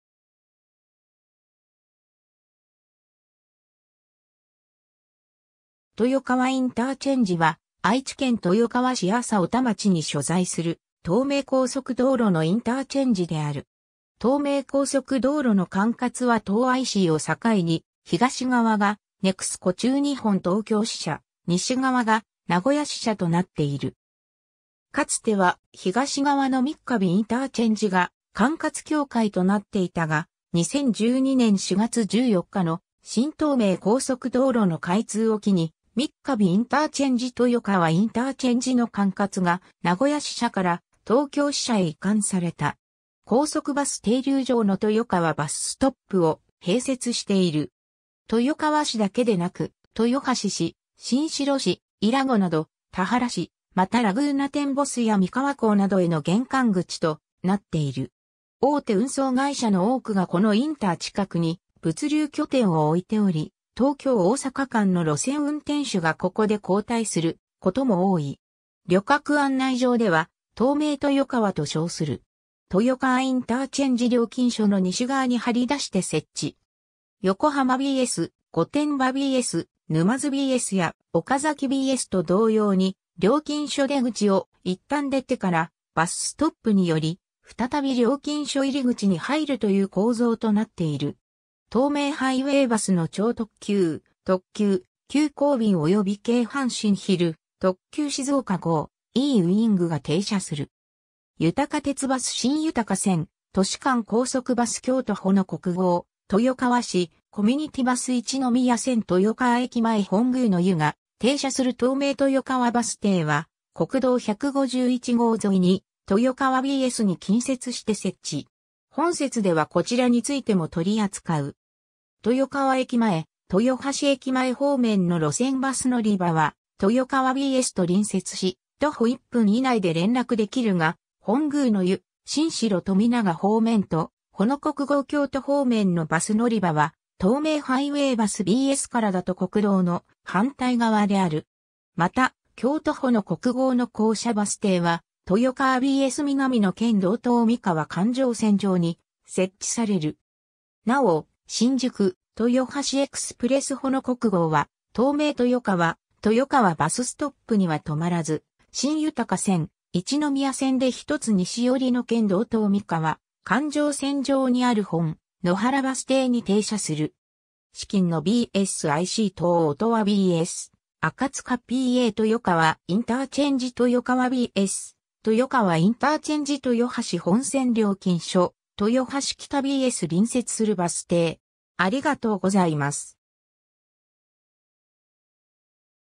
豊川インターチェンジ
豊川インターチェンジ（とよかわインターチェンジ）は、愛知県豊川市麻生田町に所在する東名高速道路のインターチェンジ (IC) である。
 東名高速道路の管轄は当ICを境に東側がNEXCO中日本東京支社（旧JH東京第二管理局）、西側が名古屋支社（旧JH中部支社）となっている（豊川ICは名古屋支社の管轄）。
かつては東側の三ヶ日ICが管轄境界となっていたが、2012年4月14日の新東名高速道路の開通を機に三ヶ日IC - 豊川ICの管轄が名古屋支社から東京支社へ移管された。
 高速バス停留所の豊川バスストップ (BS) を併設している。
 豊川市だけでなく豊橋市・新城市、伊良湖など田原市、またラグーナテンボスや三河港などへの玄関口となっている。
 大手運送会社の多くがこのインター近くに物流拠点を置いており、東京 - 大阪間の路線運転手がここで交代することも多い。
 旅客案内上では東名豊川（とうめいとよかわ）と称する。
豊川インターチェンジ料金所の西側に張り出して設置。
 横浜BS（廃止）・御殿場BS・沼津BSや岡崎BSと同様に、料金所出口をいったん出てからバスストップに寄り、再び料金所入口に入るという構造となっている。
 東名ハイウェイバスの超特急・特急・急行便および京阪神昼特急静岡号、e-wingが停車する。
 豊鉄バス 新豊線（豊橋駅前 - 新城富永）・都市間高速バス 京都ほの国号（豊橋京都線）、豊川市コミュニティバス 一宮線（豊橋駅前 - （豊鉄バス 豊川線直通） - 豊川駅前 - 本宮の湯）が停車する東名豊川バス停は、国道151号沿いに豊川BSに近接して設置。
本節ではこちらについても取り扱う。
 豊川駅前・豊橋駅前方面の路線バス乗り場は豊川BSと隣接し徒歩1分以内で連絡できるが、本宮の湯・新城富永方面とほの国号京都方面のバス乗り場は東名ハイウェイバスBSからだと国道の反対側である。
また、京都ほの国号の降車バス停は豊川BS南の県道東三河環状線上に設置される。
 なお、新宿・豊橋エクスプレス ほの国号は東名豊川/豊川バスストップには停まらず、新豊線・一宮線で1つ西寄りの県道東三河環状線上にある本野原バス停に停車する。
 至近のBS・IC等 音羽BS - 赤塚PA - 豊川IC - 豊川BS - 豊川IC - 豊橋本線料金所（チェックバリア・廃止） - 豊橋北BS 隣接するバス停